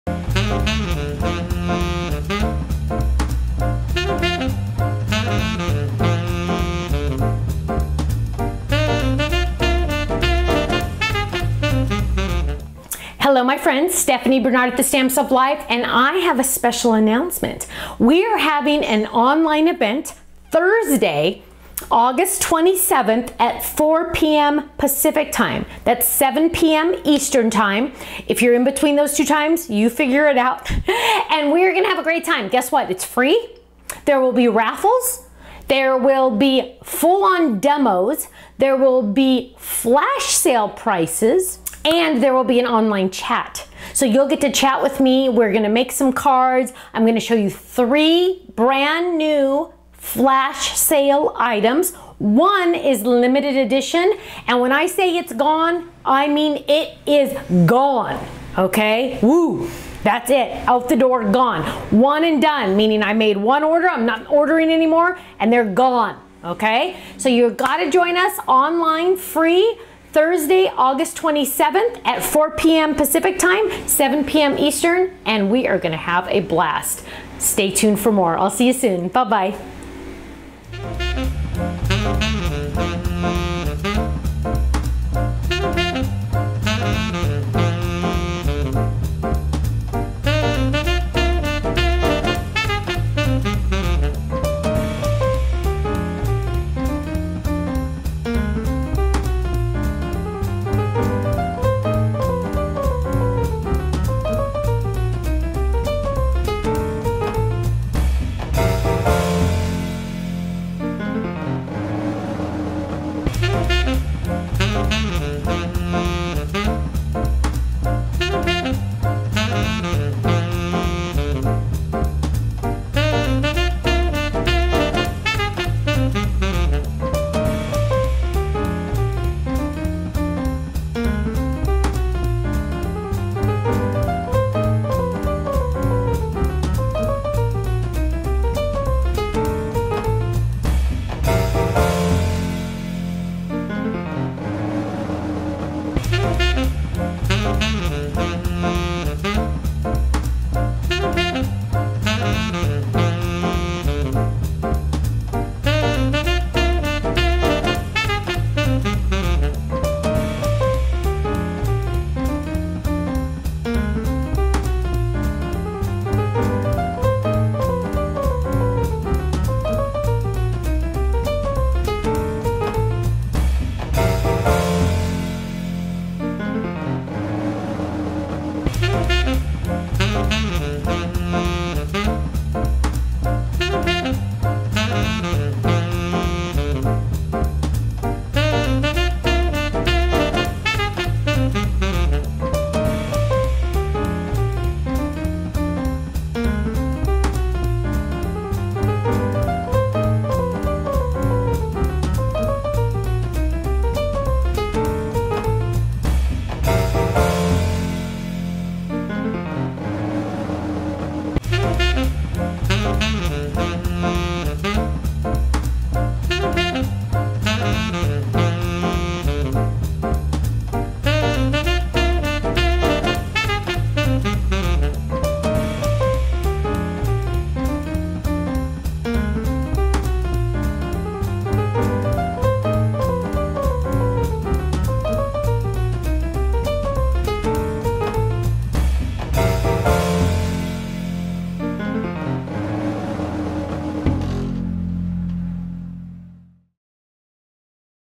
The Stamps of Life Online Crafting Celebration Thursday August 27th 4PM PST/7PM EST
Don't miss The Stamps of Life Online Crafting Celebration Thursday August 27th 4PM PST/7PM EST with Stephanie Barnard and The Stamps of Life team! This will be a FREE and FUN event for everyone!
Stephanie will demo some new products, there will be flash sales, limited edition items, chat, and drawings for prizes! Mark your calendar, save the date, and be sure to tell your friends!
You'll be able to watch on YouTube, Facebook, The Stamps of Life website, here's the links: